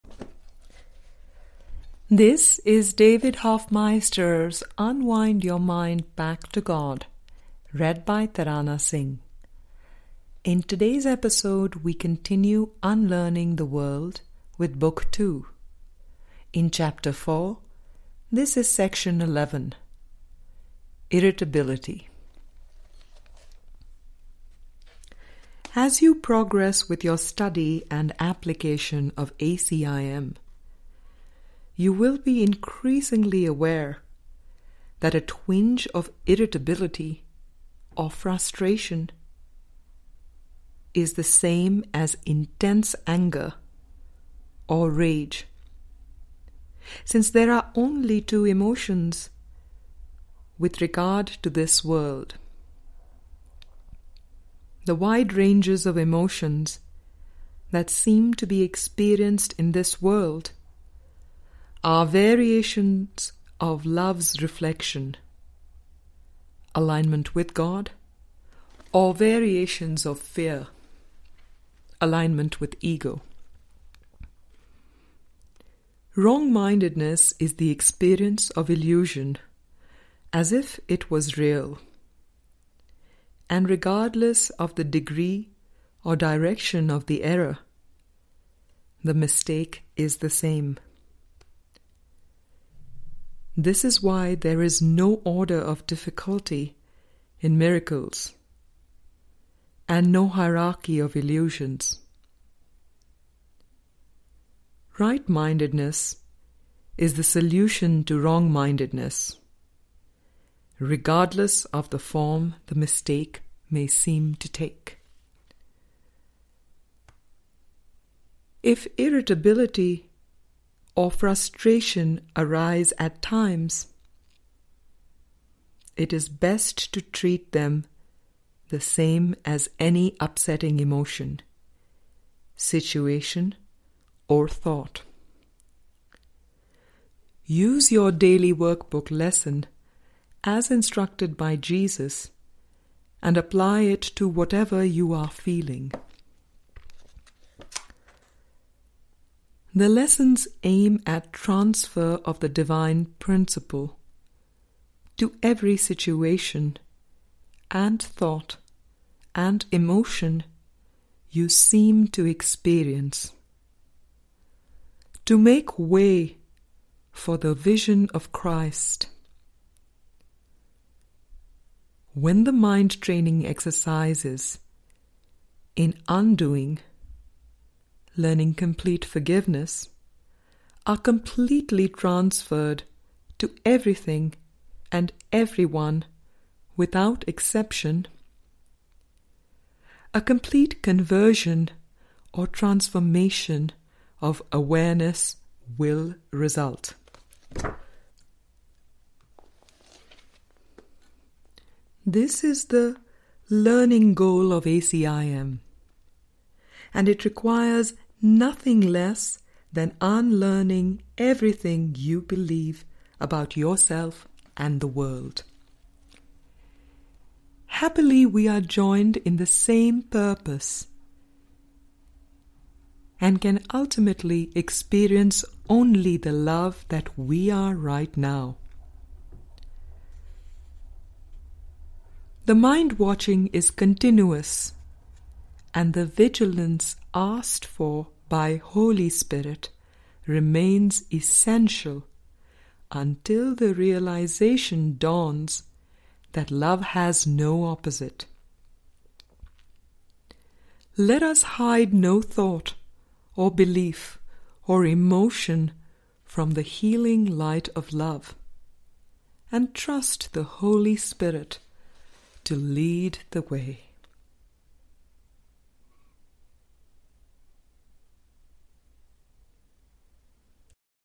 Unwind Your Mind Book. 2 Ch. 4 Sec. 11 - Irritability - David Hoffmeister ACIM
This reading includes the eleventh section of Unwind Your Mind Chapter. 4 ( Irritability )  from Book Two: Unlearning the World.
This is the one hundred and twenty fifth of several upcoming episodes featuring readings of this profound work of consolidated conversations between ACIM teacher David Hoffmeister and his students.